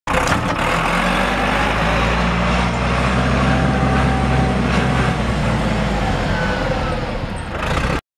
1987 VOLVO L50 For Sale
Title: 1987 VOLVO L50 For Sale

Description: Forks shown in picture are not included. Weight 17,857 lbs, Good runner in working condition.